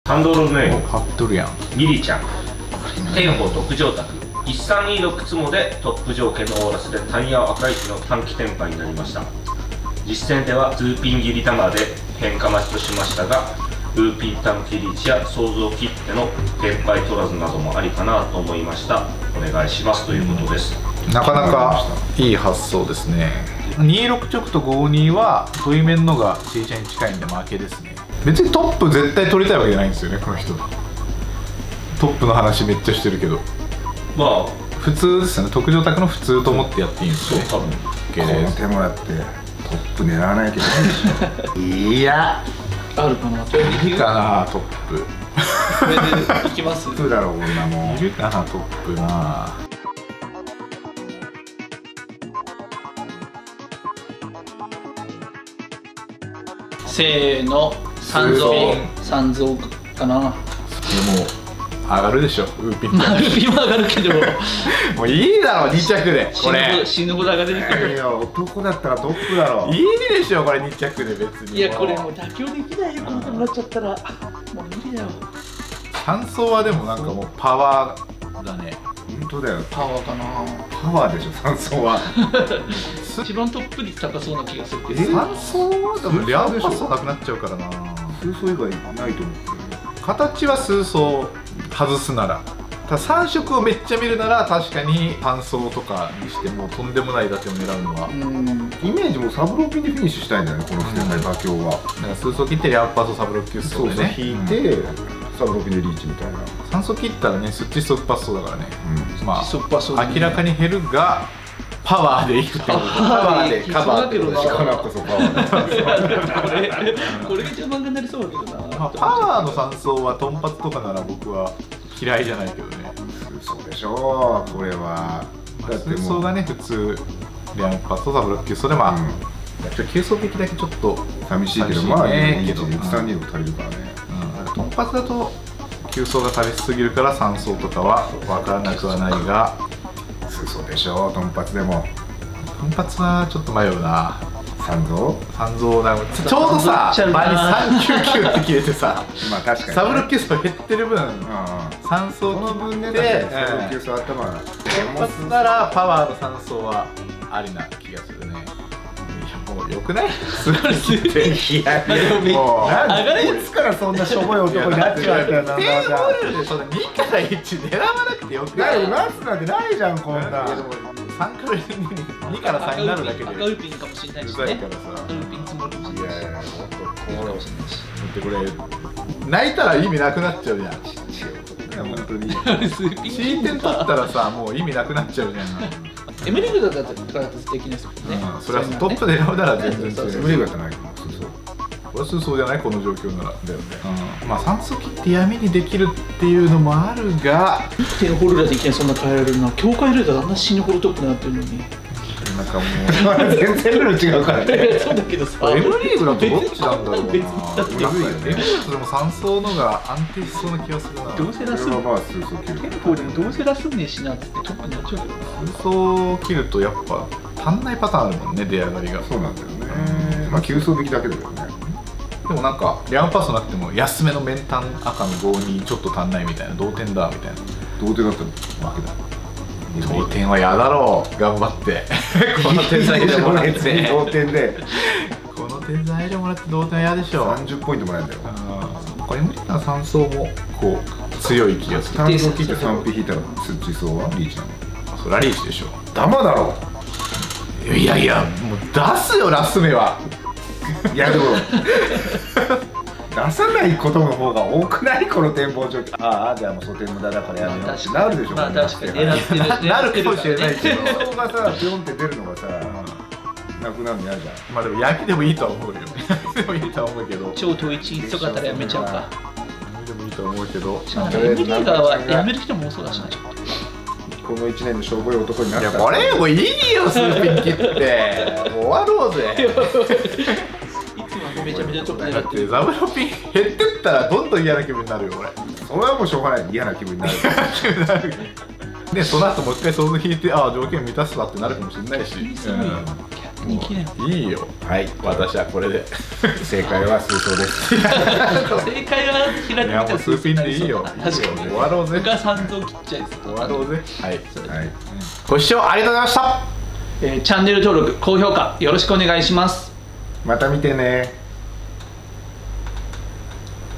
何切る超会議その117@日本プロ麻雀協会
【月・金更新】
視聴者の方からいただいた何切る問題に、日本プロ麻雀協会
現雀王・仲林圭

がお答えします！